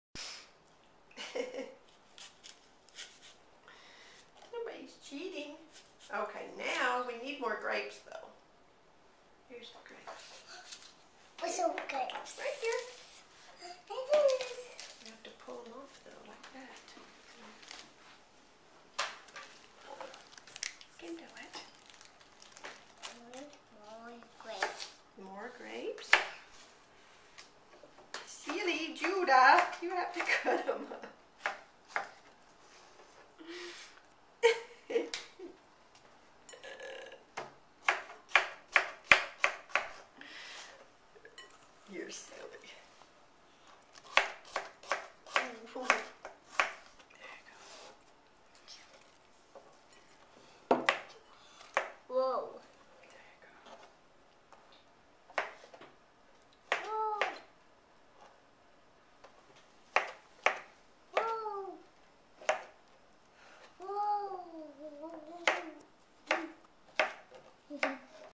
Judah & Nonnie; Fruit Salad 2
Judah helps Nonnie make fruit salad, part deux; Recorded on July 22, 2010 using an HD  Flip Video camcorder.